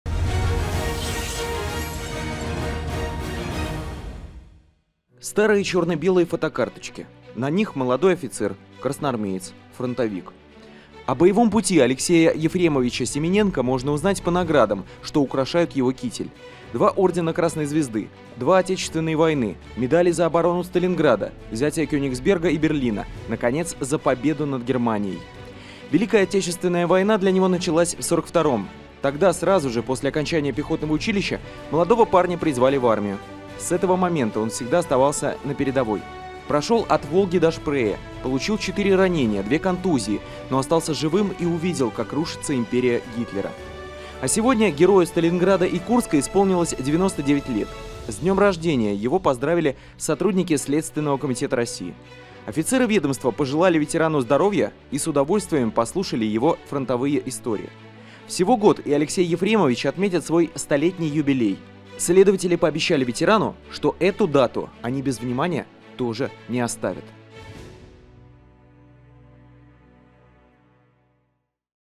В Курской области сотрудники СК России поздравили ветерана ВОВ с Днем рождения
Сегодня свой день рождения отмечает ветеран Великой Отечественной войны, подаривший всем нам чистое небо над головой Алексей Ефремович Семененко. Он проживает в городе Курске, является ветераном Великой Отечественной войны, принимал участие в Сталинградской битве. Ему исполнилось 99 лет.
В этот особенный день сотрудники регионального ведомства навестили Алексея Ефремовича и поздравили его с днем рождения.
Именинник, в свою очередь охотно пообщался с офицерами, рассказав несколько историй военного и послевоенного времени.
Дочь героя – Лидия Алексеевна, поделилась рассказами, фотографиями времен Великой Отечественной войны.
Она рассказала, что в 1942 году, после окончания пехотного училища  отца призвали в армию. Служил он в стрелковой дивизии, занимавшей оборону на правом берегу Дона. За операцию дивизия, получила орден боевого Красного Знамени. Дивизия была направлена на места, где шли самые тяжелые бои.
В составе этой дивизии папа воевал всю войну, пройдя путь от Сталинграда до Берлина. Всю войну провел на передовой. Получил 4 ранения, две контузии, говорит Лидия Алексеевна.
За умелые отважные действия в боях за Родину, он награжден двумя орденами «Красной Звезды», двумя орденами «Отечественной войны», медалями «За оборону Сталинграда», «За победу над Германией в Великой Отечественной войне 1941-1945 гг.», «За взятие Кенигсберга», «За взятие Берлина».
Сотрудники следственного управления пожелали ветерану крепкого здоровья, бодрости духа, неиссякаемой энергии, долгих лет жизни, и конечно же, искренне поблагодарили Алексея Ефремовича за вклад, внесенный в Победу над фашизмом.
Ветеран и его дочь поблагодарили сотрудников за внимание, подарки и положительные эмоции. В конце встречи они пожелали всем чистого неба над головой, мира, счастья и благополучия.